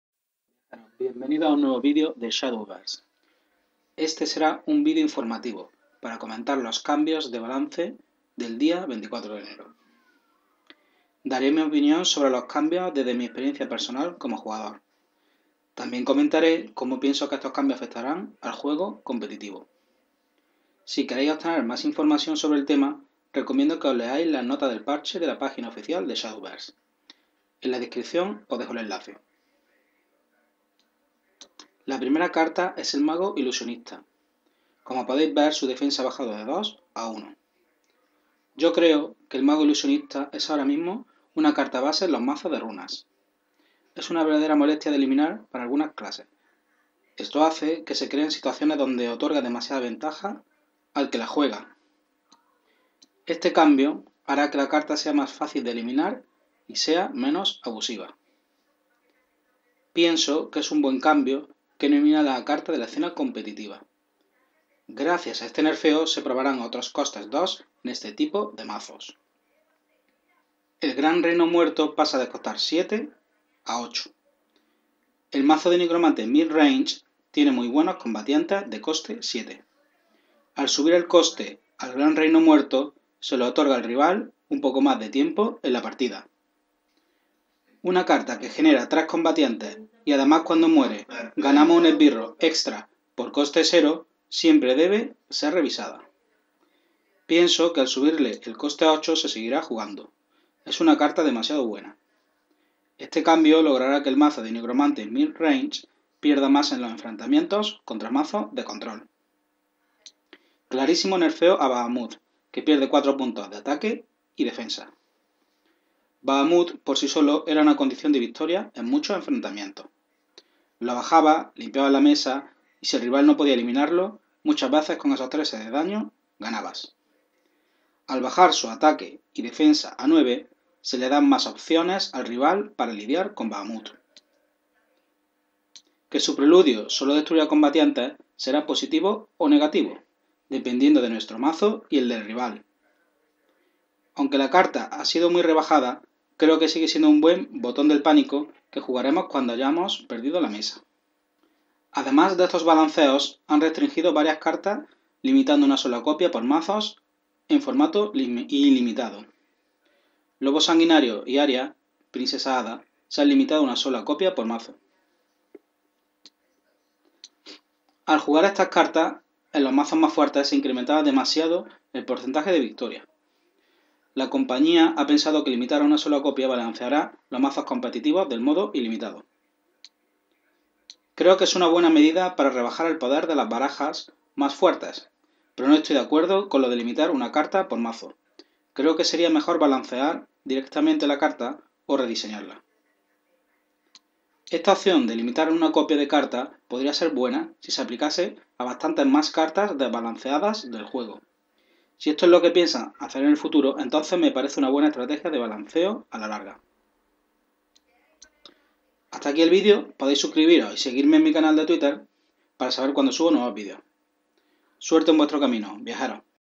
Shadowverse en Español. Parche de Balanceo 24/01/2018. Breve explicación y mi opinión.
En este vídeo os explicare el parche del día 24/01/2018 comentando las cartas que han cambiando y dando mi opinión sobre ello.
Un saludo espero que el vídeo os sirva.